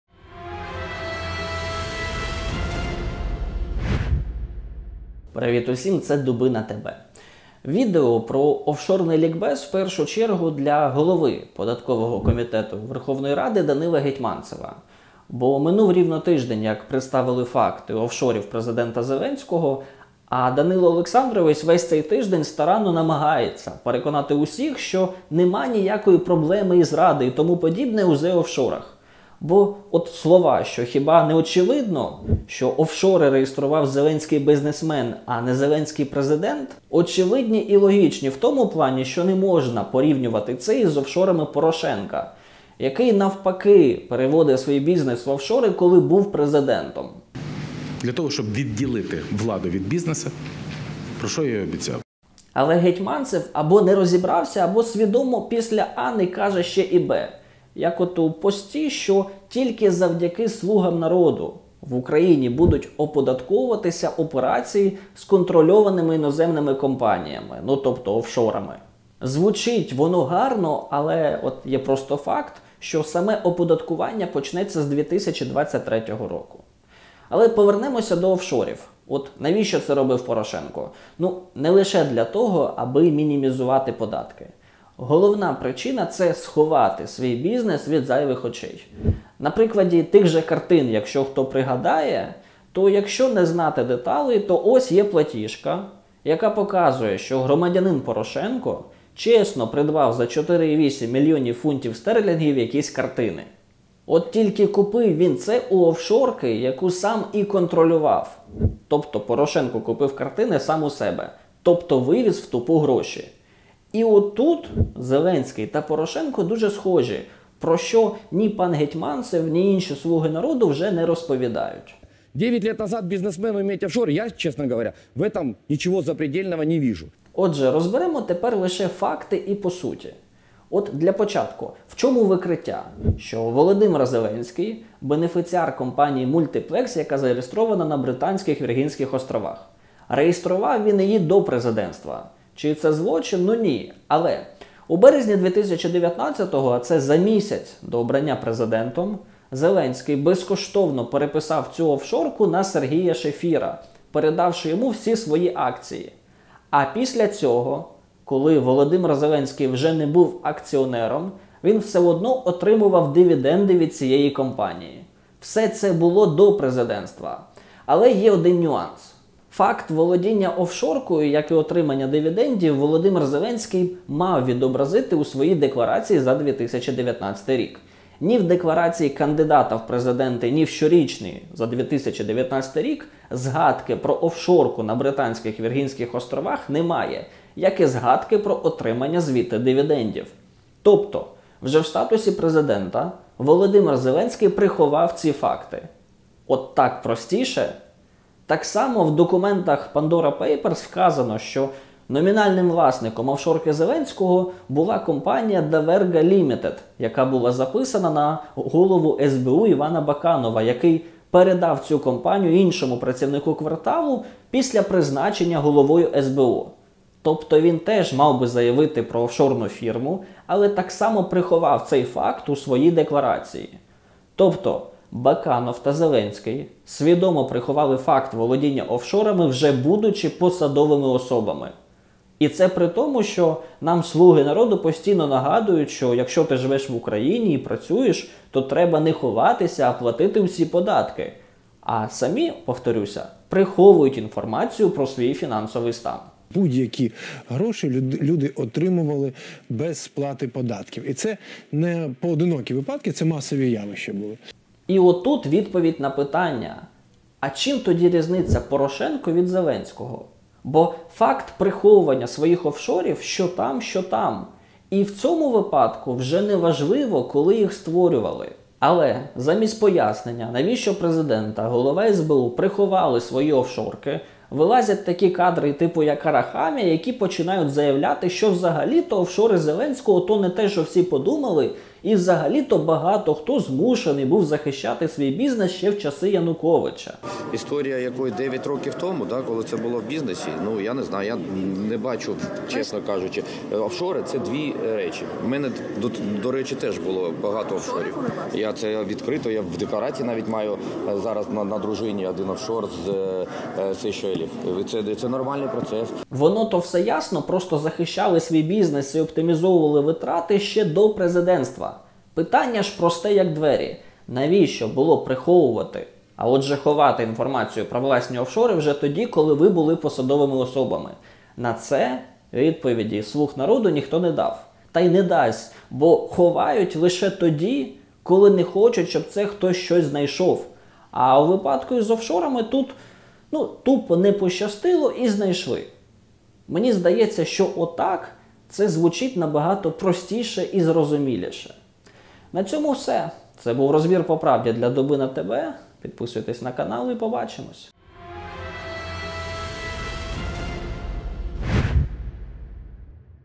ЗеОффшоры не миф: ликбез для Гетьманцева
Владимир Зеленский продолжает отмалчиваться о своих оффшорах. Тем временем слуги народа отчаянно защищают президента.

Главный вывод всех говорунов: это другое!

На самом деле нет. Объясняем почему оффшоры Зеленского не сильно отличаются от оффшоров Порошенко.